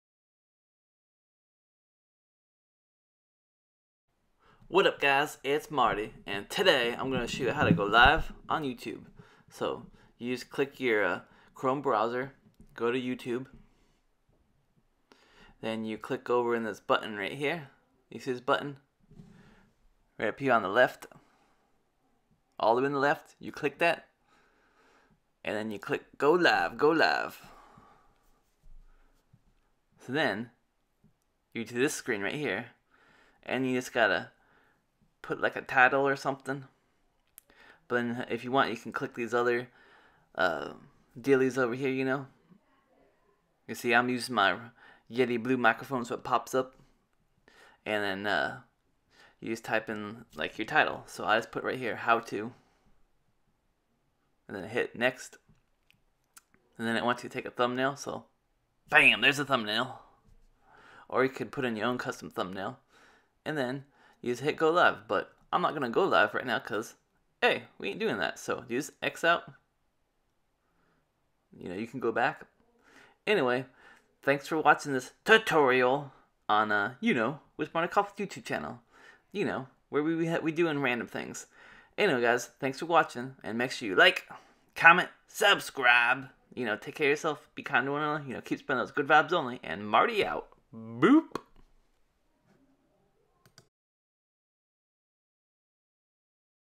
How to go Live on YOUTUBE | tutorial
This is a tutorial of how to go live on YouTube